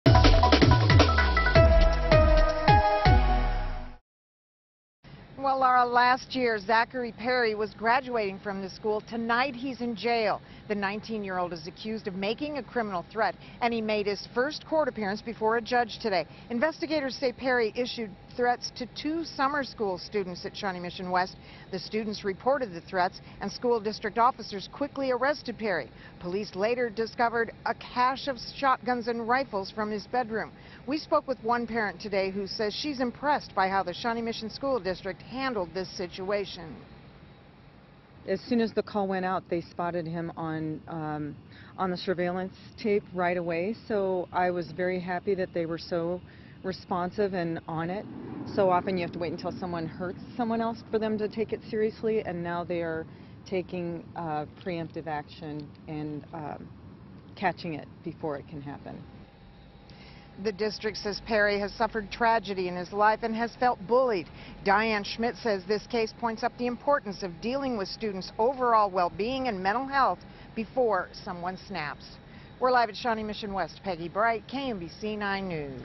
Teen facing charges after threats made at Shawnee Mission West
A 19-year-old is accused of making threats against students at Shawnee Mission West High School.